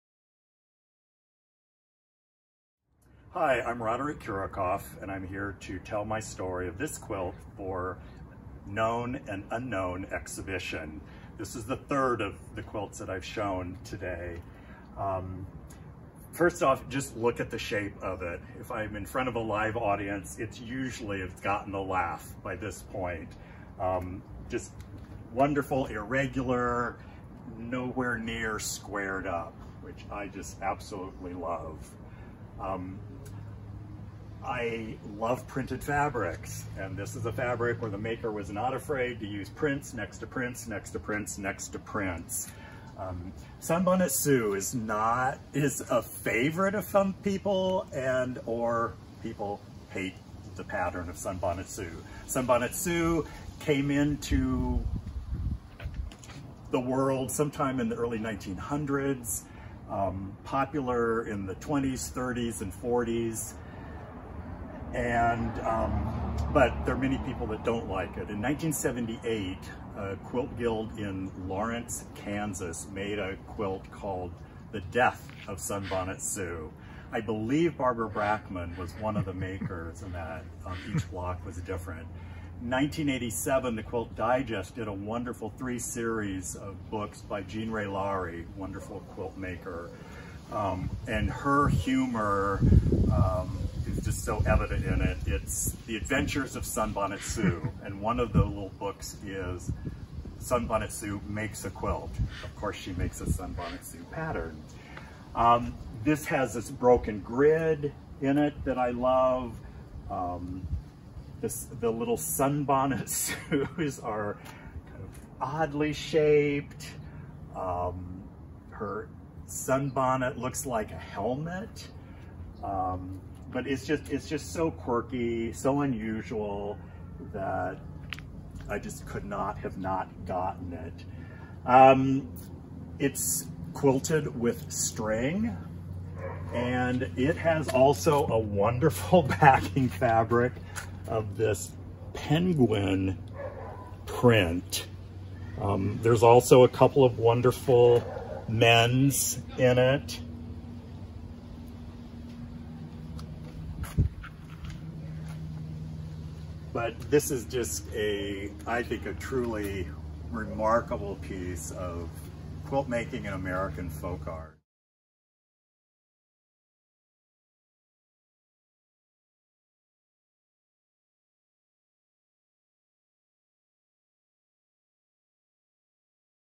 Go Tell It at the Quilt Show! interview with Roderick Kiracofe
Roderick Kiracofe recorded this Go Tell It video about this "Sunbonnet Sue Variation" quilt by an unknown maker for the "Known and Unknown: Revealing Quilt Stories" exhibition, part of the 2020 Quilters Take a Moment event. This video and exhibition were made possible by a grant from the Coby Foundation.

"Go Tell It at the Quilt Show!" is a Quilt Alliance project designed to capture the stories of quilts where quiltmakers gather.